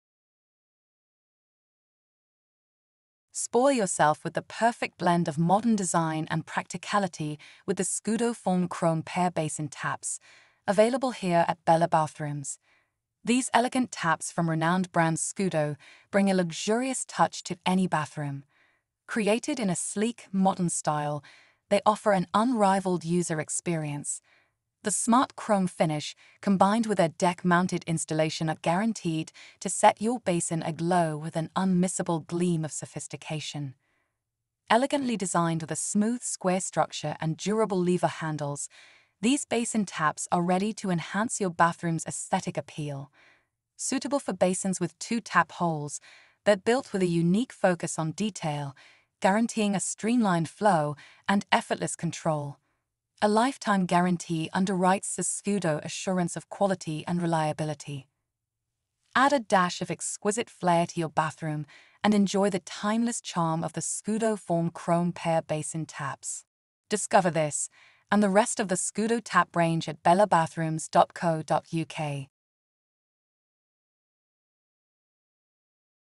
Scudo Forme Chrome Pair Basin Taps - Available at Bella Bathrooms
Spoil yourself with the perfect blend of modern design and practicality with the Scudo Forme Chrome Pair Basin Taps. These elegant taps from renowned brand Scudo bring a luxurious touch to any bathroom. Created in a sleek, modern style, they offer an unrivalled user experience. The smart chrome finish combined with their deck mounted installation are guaranteed to set your basin aglow with an unmissable gleam of sophistication.

Elegantly designed with a smooth square structure and durable lever handles, these basin taps are ready to enhance your bathroom's aesthetic appeal. Suitable for basins with two tap holes, they're built with a unique focus on detail, guaranteeing a streamlined flow and effortless control. A lifetime guarantee underwrites the Scudo assurance of quality and reliability.

Add a dash of exquisite flair to your bathroom and enjoy the timeless charm of the Scudo Forme Chrome Pair Basin Taps.

Finished in high quality Chrome
Basin taps
Comes in pair
Operating Pressure: 0.2 bar

Dimensions:
Height: 103.5mm
Width: 34mm
Length: 99.5mm

Bella Bathrooms Ltd
Unit 10
Bowburn South Industrial Estate
Bowburn
Durham
DH6 5AD